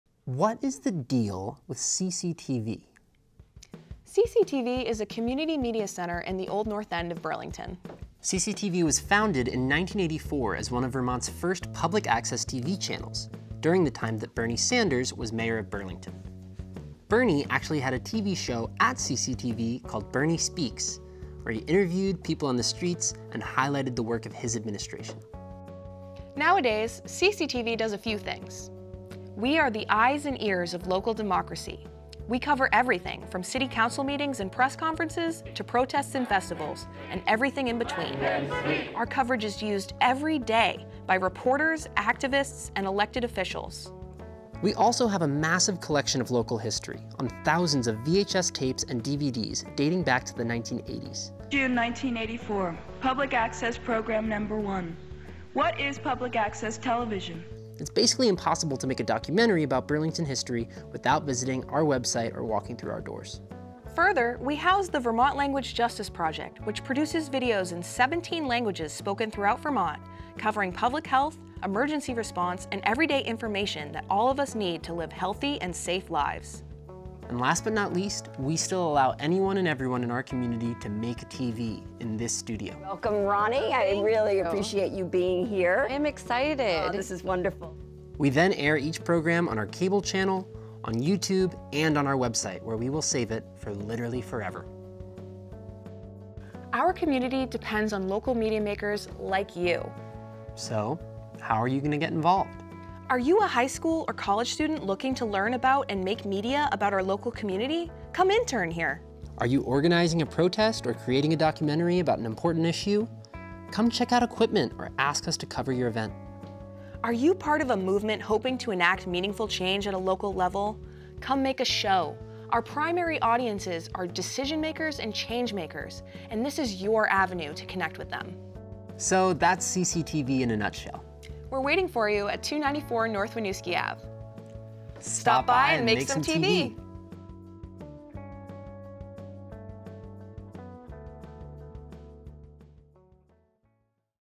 What's the Deal with CCTV?
Bobby Lussier and Emily Brewer explain CCTV in a nutshell!